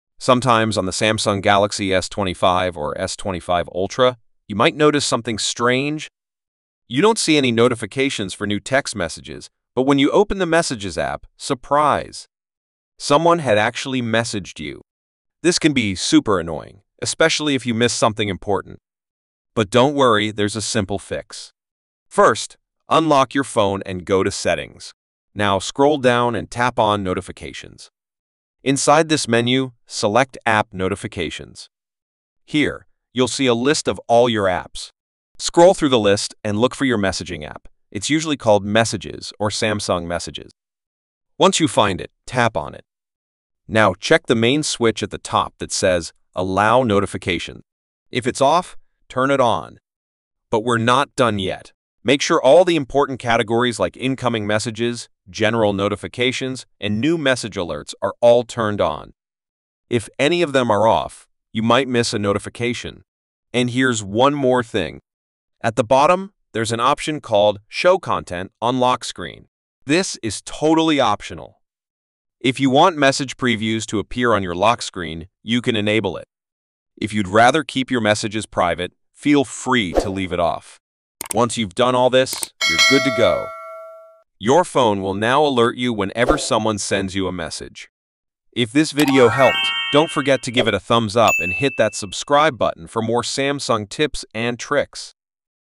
How to Fix Text Message Notifications Not Showing on Samsung Galaxy S25 / S25 Ultra
Are you missing important text messages on your Samsung Galaxy S25 or S25 Ultra because notifications aren't showing up?
In this step-by-step video, I’ll show you exactly how to enable and fix SMS notifications so you never miss a message again!

✅ Learn how to:

Turn on message alerts

Enable lock screen previews

Fix the "no notification" bug in seconds

Whether you're using the S25 or the S25 Ultra, this quick guide will solve your problem in under 2 minutes!